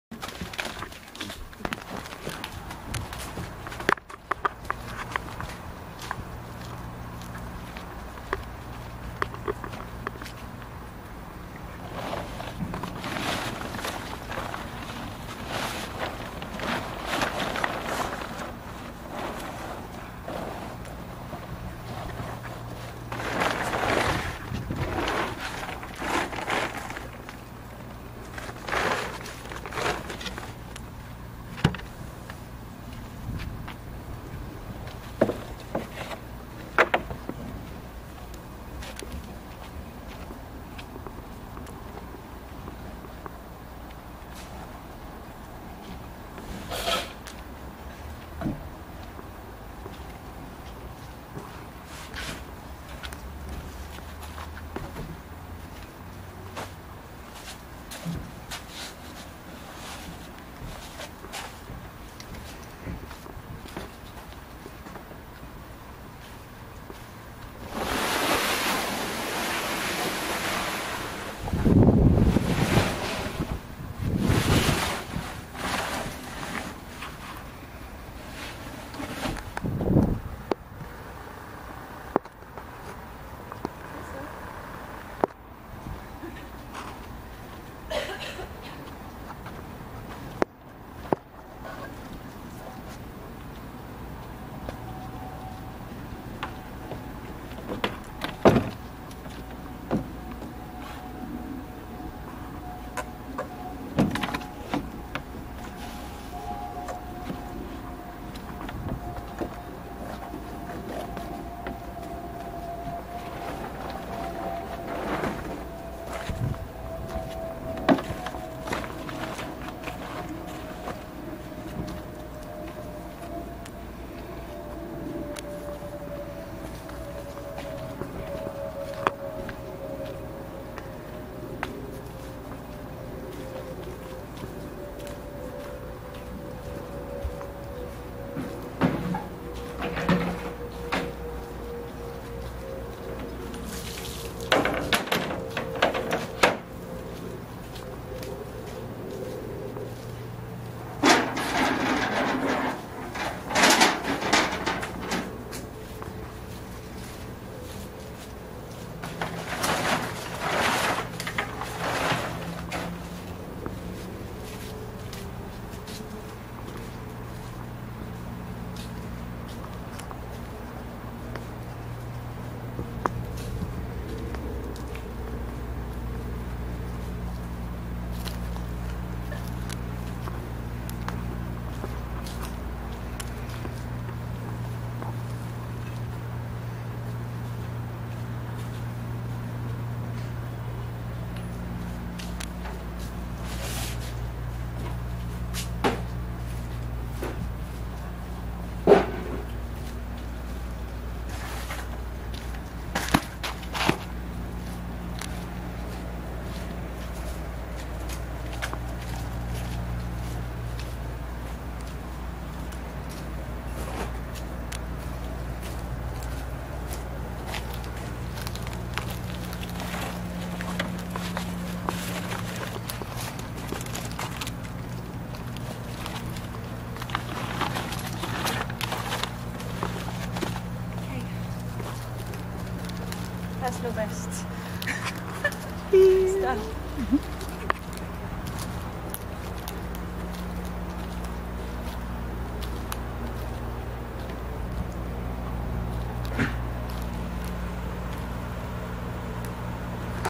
set up nursery 2014
Setting up the outdoor area each morning takes just a few minutes.